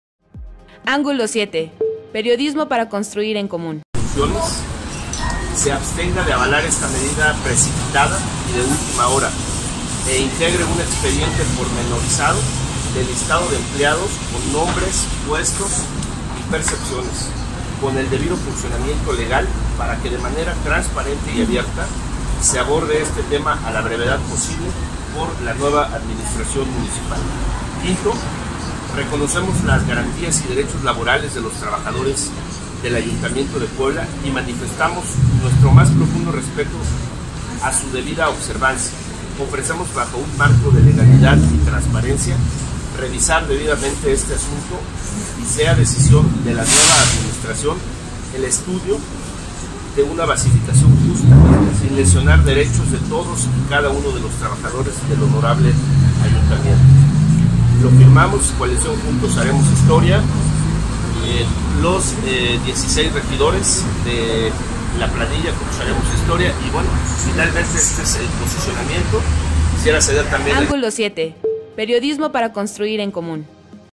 Regidores electos piden a salientes no dar basificación por “compadrazgo”
Regidores electos del próximo Cabildo de Puebla pidieron a sus homólogos salientes no aprobar este viernes la basificación de 84 trabajadores del ayuntamiento sin antes analizar los perfiles.